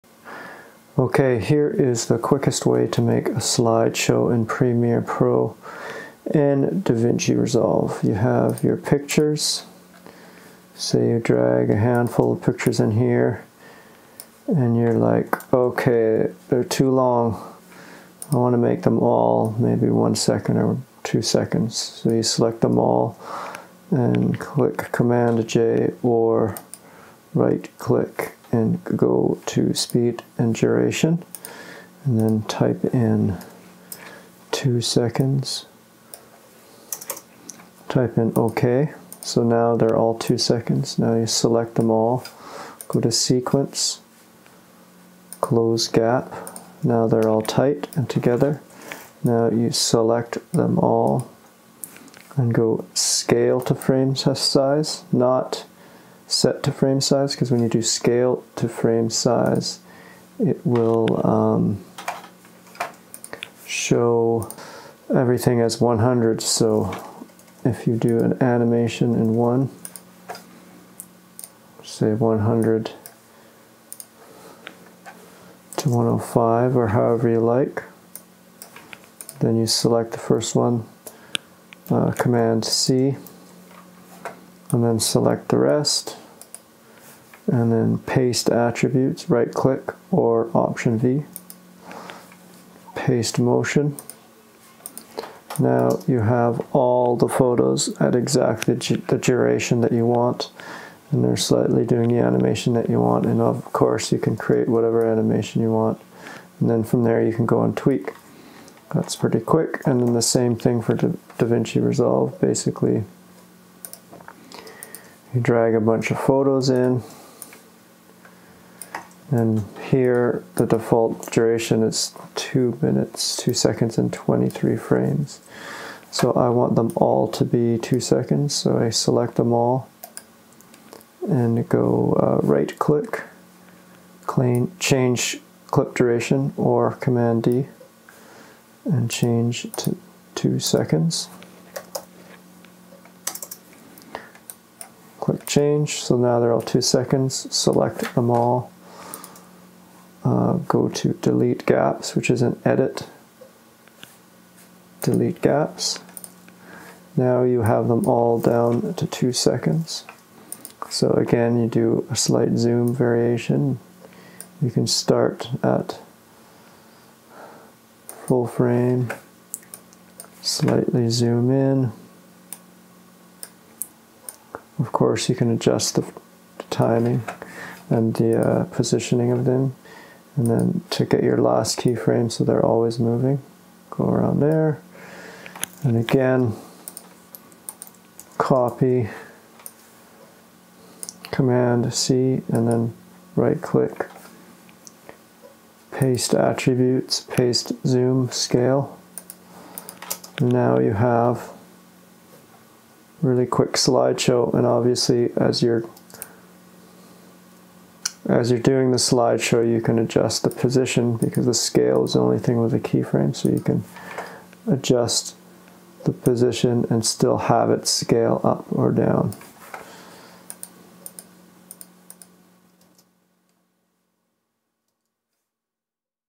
What's the fastest way to make a photo slideshow - DaVinci or Premiere
The fastest way to make a photo slideshow in DaVinci or Premiere

00:00 in Premiere
01:44 in Resolve